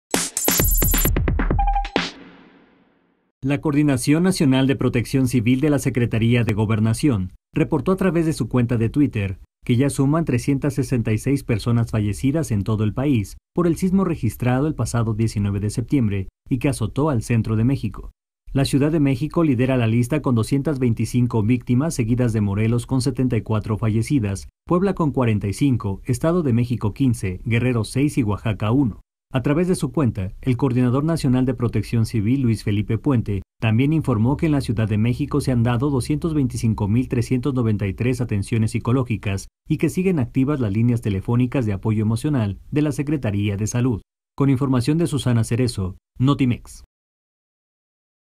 Sube a 366 número de fallecidos por terremoto en México
A través de su cuenta de twitter, la Coordinación Nacional de Protección Civil actualizó la cifra de víctimas que perdieron la vida en el sismo del 19S.